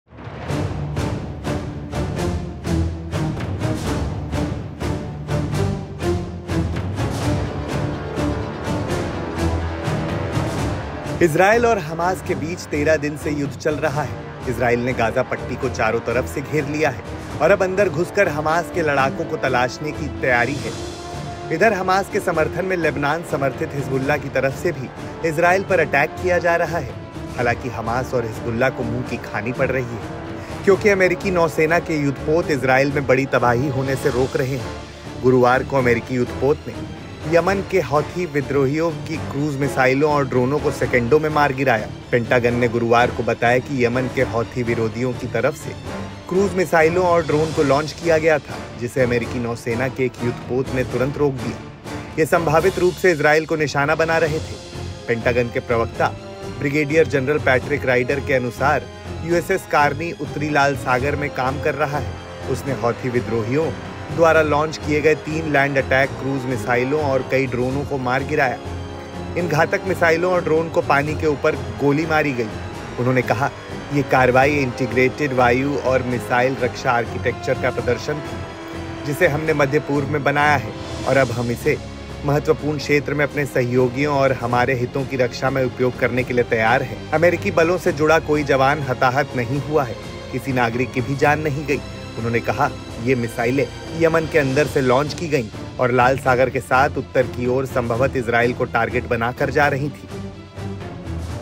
AAJTAK 2 | 'ISREAL पर हमले की धमकी दे चुके हौथी विद्रोही', लेबनान में हिजबुल्लाह के ठिकानों पर हमला
इजरायल और हमास के बीच 13 दिन से युद्ध चल रहा है. इजरायल ने गाजा पट्टी को चारों तरफ से घेर लिया है और अब अंदर घुसकर हमास के लड़ाकों को तलाशने की तैयारी है. इधर, हमास के समर्थन में लेबनान समर्थित हिजबुल्लाह की तरफ से भी इजरायल पर अटैक किया जा रहा है. हालांकि, हमास और हिजबुल्लाह को मुंह की खानी पड़ रही है.
The war between Israel and Hamas has been going on for 13 days. Israel has surrounded the Gaza Strip from all sides and is now preparing to enter inside and search for Hamas fighters. Here, in support of Hamas, Israel is also being attacked by Lebanon-backed Hezbollah. However, Hamas and Hezbollah are facing losses.